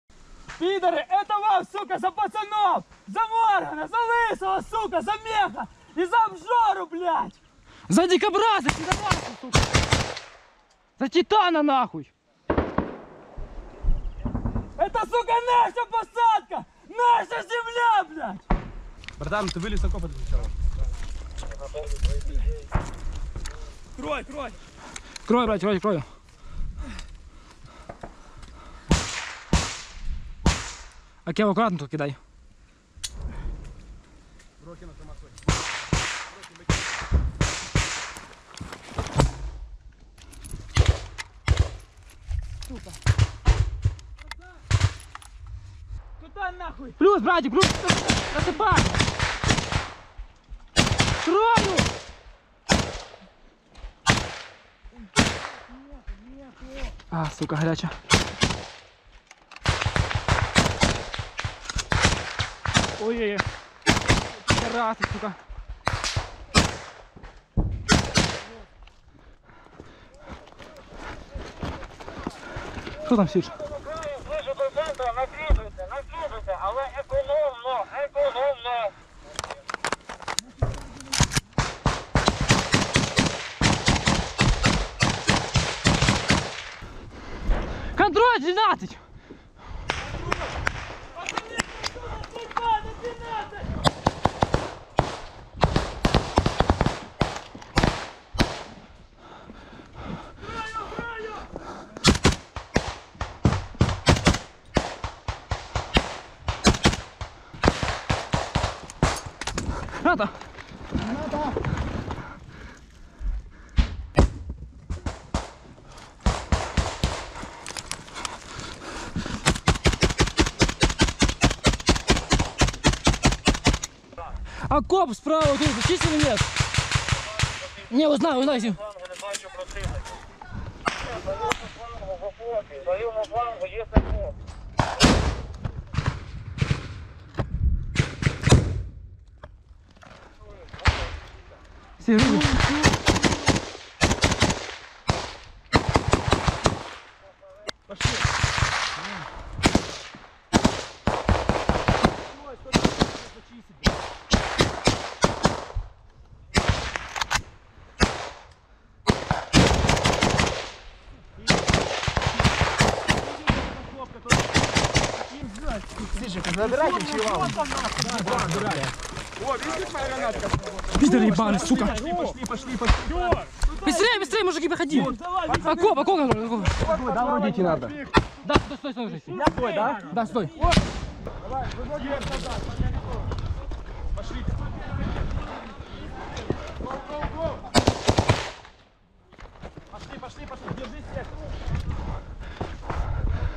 Штурмові бої у траншеях та ліквідовані окупанти
Мотивація в помсті. Бійці 3-ї роти 1-го механізованого батальйону  зачищають окопи від ворога та закріплюються у них для подальшого ведення вогню.

Це наступ Третьої штурмової бригади на південно-західних околицях Бахмута.